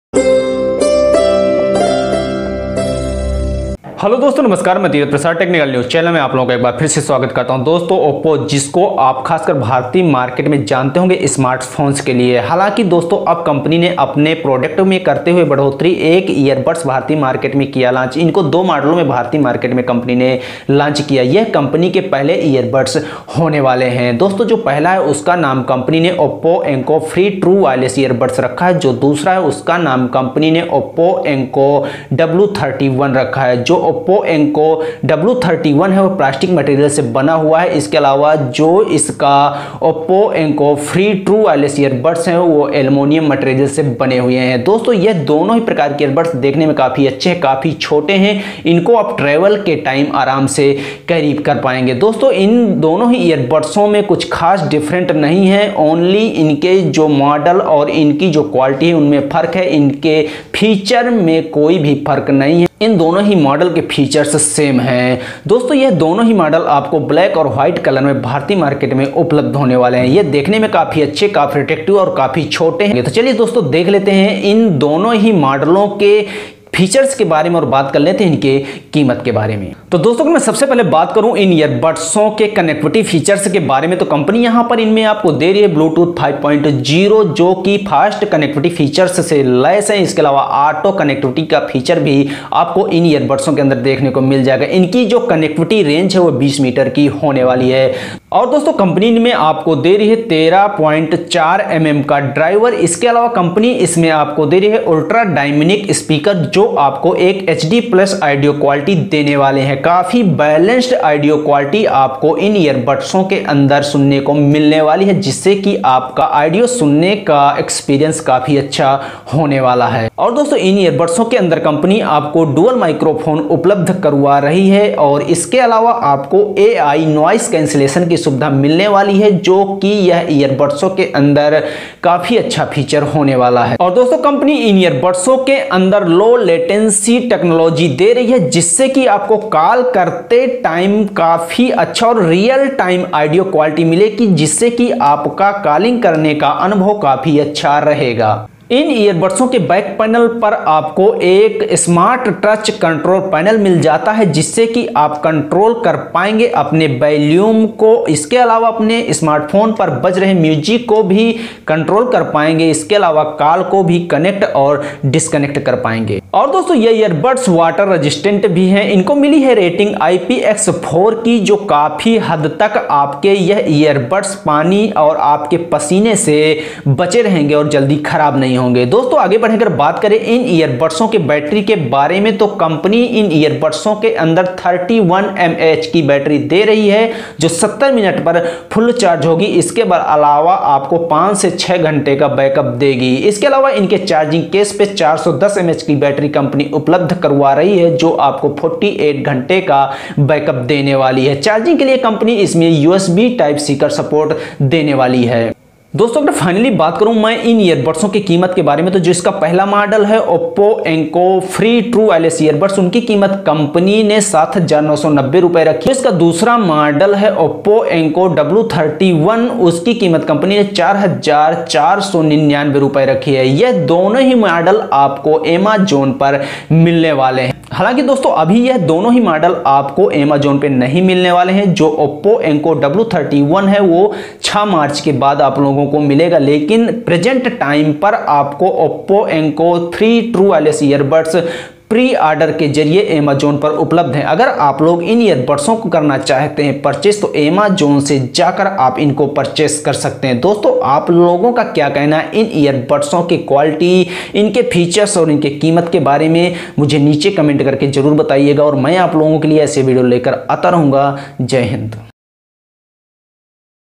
OPPO Enco Free and Oppo Enco W31 Review Hindi
Oppo Enco Free and oppo W31 ओप्पे के वायरसेल बड्स को किसी भी स्मार्टफोन से ब्लूटूथ के जरिए कनेक्ट किया जा सकेगा। इसमें अल्ट्रा डायनमिक स्पीकर्स दिए गए हैं, जो ड्यूल मैग्नेटिक सर्किट का इस्तेमाल करते हैं। साथ ही इसमें AI नॉइस कैंसिलेशन कॉल्स के लिए दिया गया है। साथ ही इसमें दो इयरटिप्स ऑप्शन यूजर्स को दिए जाएंगे। कंपनी का कहना है कि इनपर 5 घंटे का ऑडियो प्लेबैक यूजर्स को मिलेगा। इन ट्रूली वायरसेल खास केस के साथ Two  कलर ऑप्शंस में उतारा गया है। Enco Free की कीमत 7,990 रुपये रखी गई है और इसकी सेल 4 मार्च से शुरू होगी। और enco W31 की कीमत 4499 रुपए हैं कनेक्टिविटी के लिए कंपनी ने इनमें ब्लूटूथ 5.0 का सपोर्ट दिया है